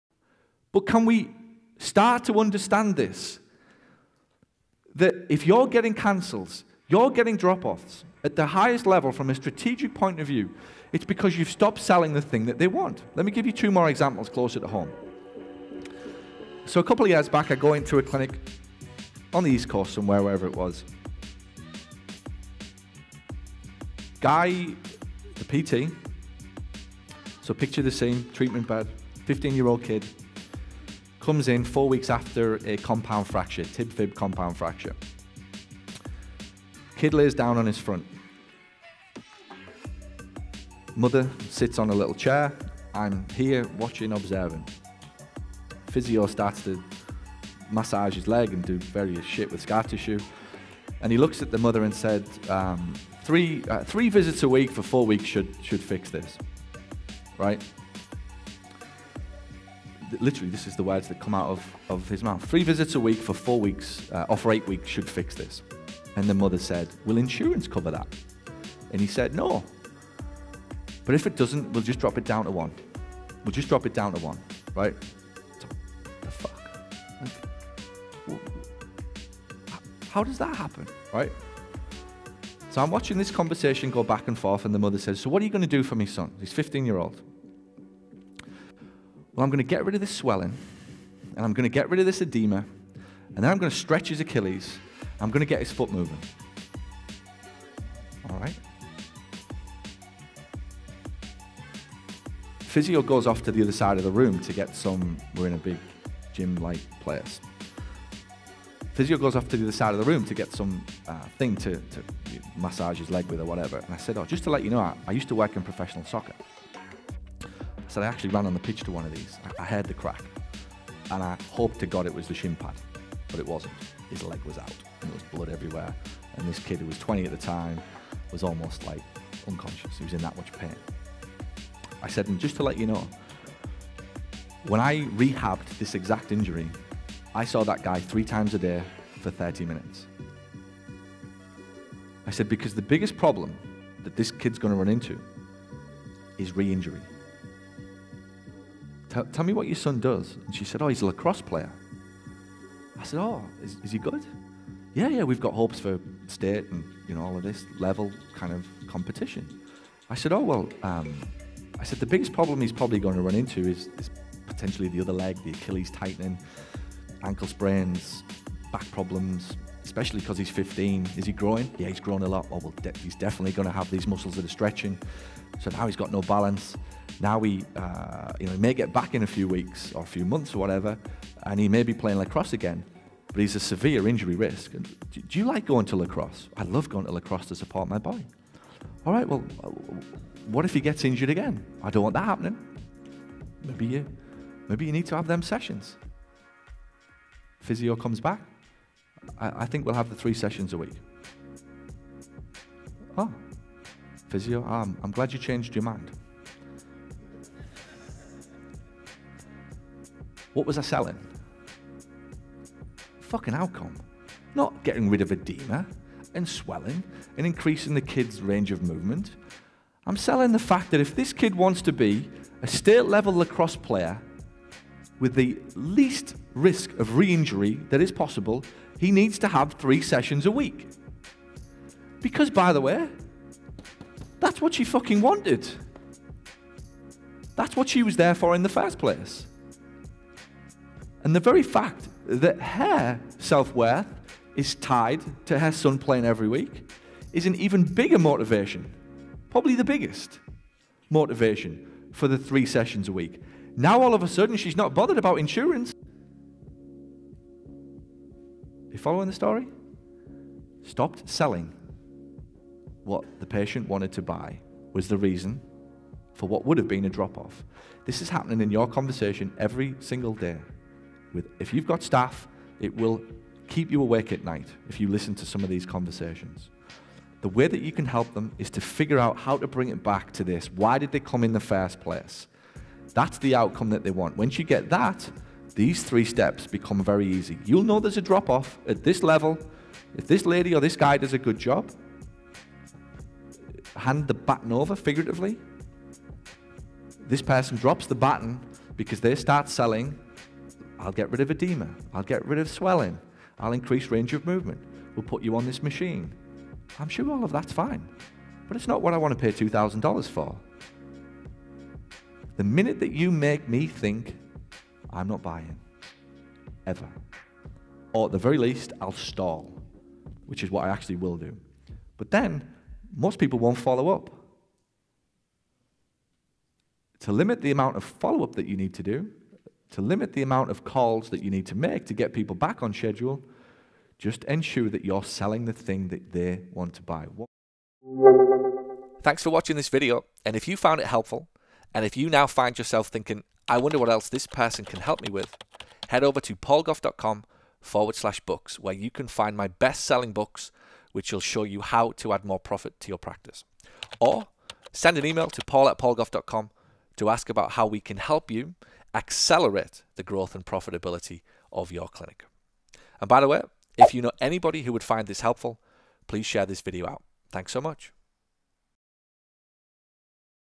Do You Struggle With Drop-Offs? | Physio Therapist Training By Paul Gough
If you have patients that are constantly ending their plans of care prematurely, there is a very good chance you, or someone in your clinic is doing this wrong... Watch this video to find out what you can do to fix this!